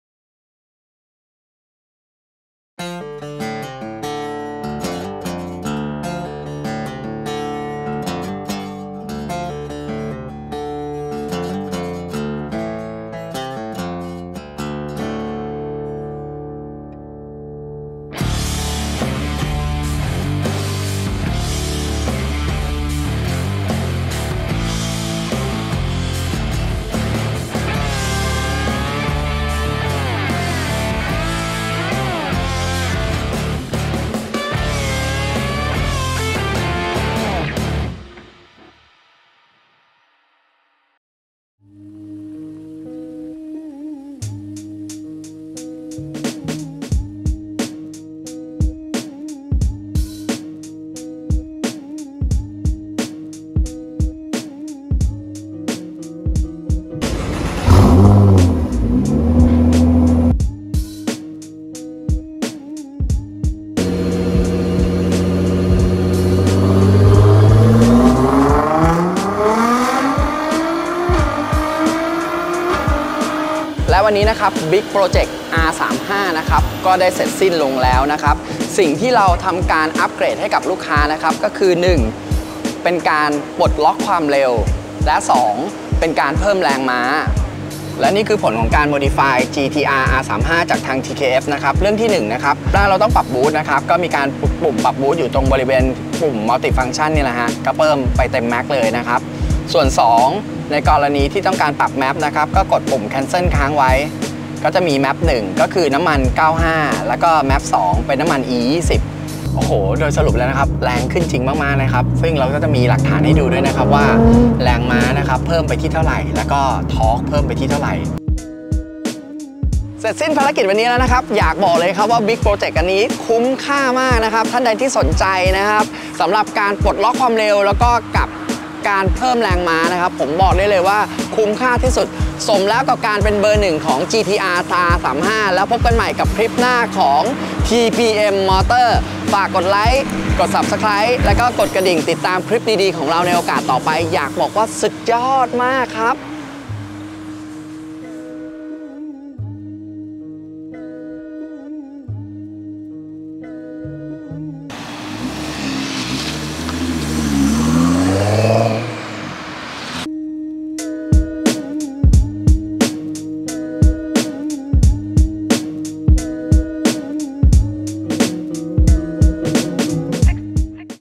TPM EP.85 : พาเจ้า GT-R ไป Remap
โปรแกรม UPGRADE GT-R35

ทำทั้งทีต้องทำให้ถึง TPM Motors
ผู้นำเข้า GT-R เบอร์หนึ่ง ร่วมกับ
TKF Racing อู่ GT-R ระดับเทพ

จัดสเตปเทพชุดใหญ่ให้กับลูกค้า
GT-R R35 ที่อยากข้ามขีดจำกัด
ของรถด้วยการโปรแกรมปรับจูนแรงม้า แรงบิด และการปลดล็อคความเร็ว โดยช่างผู้ชำนาญ
การเฉพาะด้าน จนได้ผลลัพธ์
การขับขี่ที่ดี และแรงขึ้นดังต่อไปนี้

-ปลดล็อคความเร็วเกิน 180 Kph
-แรงม้าจาก 555 hp เป็น 587 hp
-แรงบิด  636 N-m เป็น 837.49 N-m

อยากแรงแต่ปลอดภัย มั่นใจ